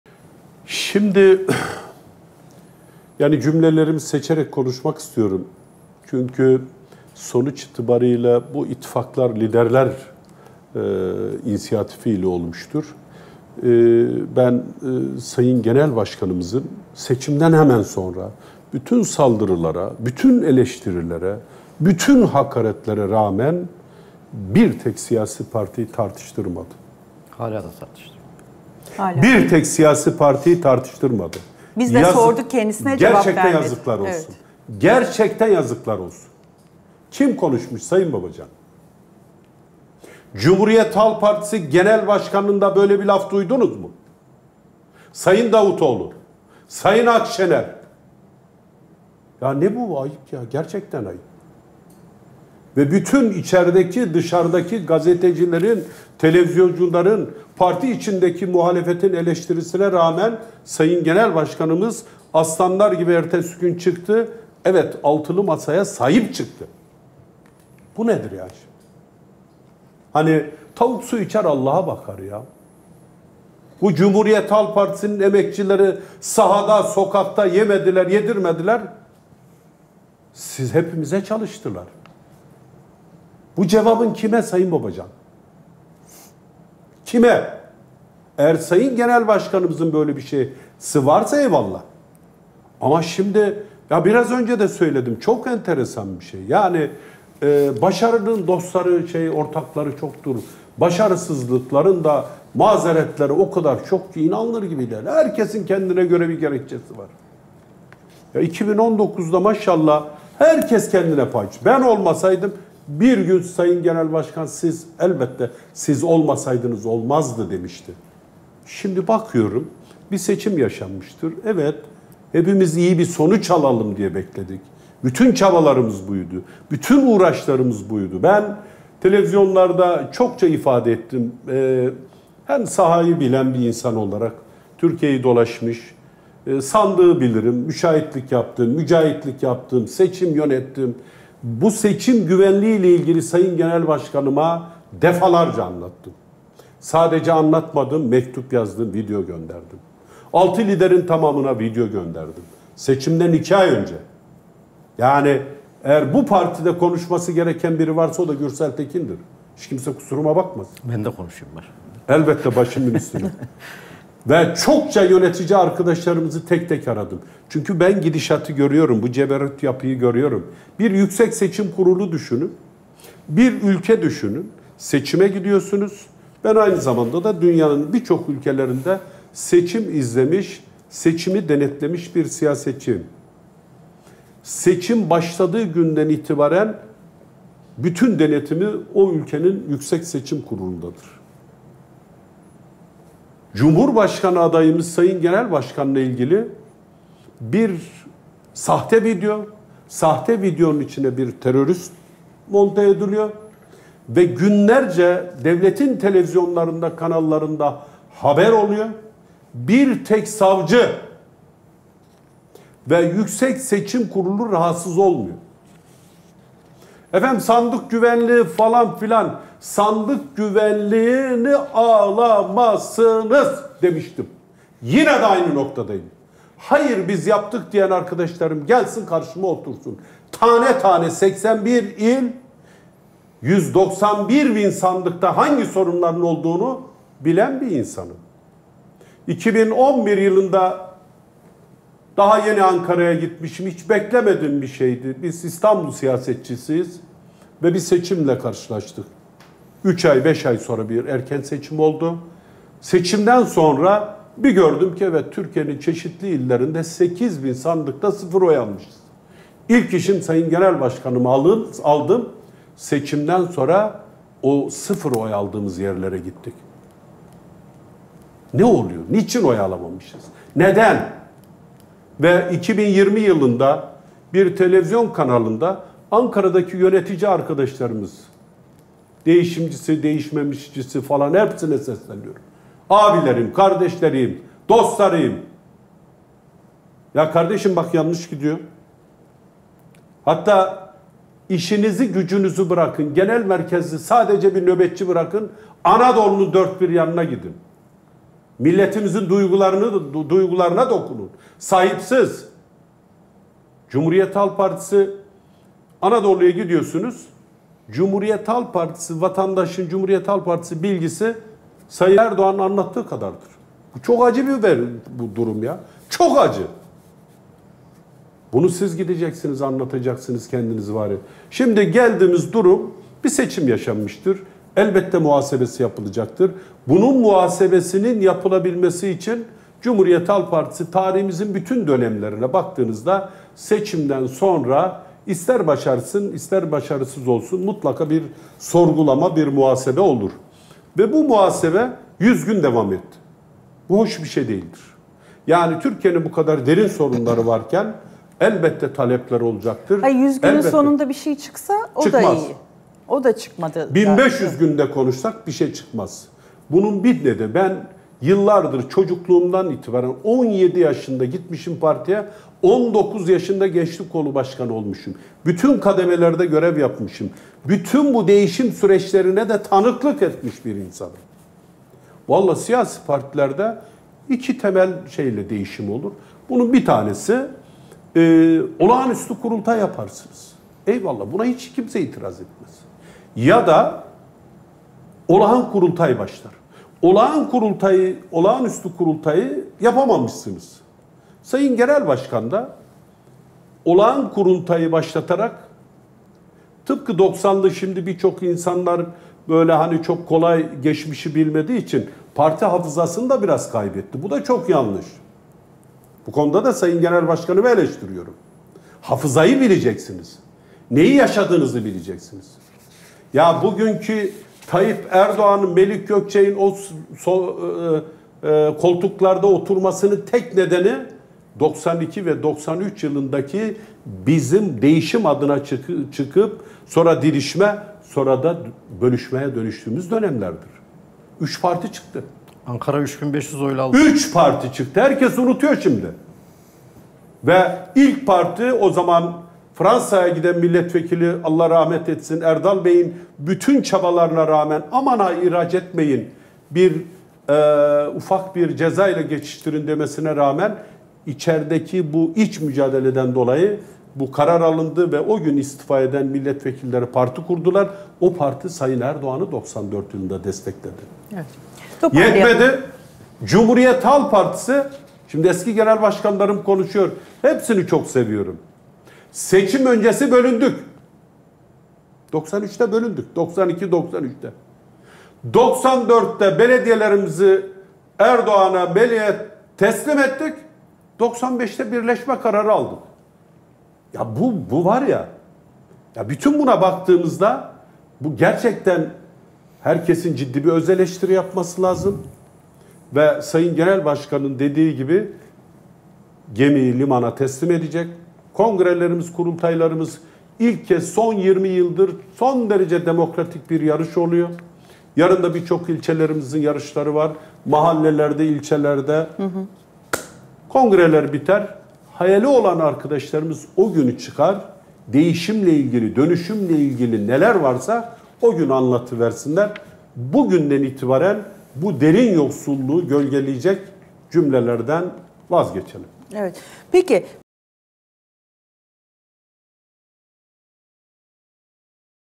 Gürsel Tekin Ali Babacan'ı Ağır Topa Tuttu! 'Yazıklar Olsun'
Digiturk: 60
D-smart: 93
Turkcell TV Plus: 69
Tivibu: 69
Türksat Kablo TV: 76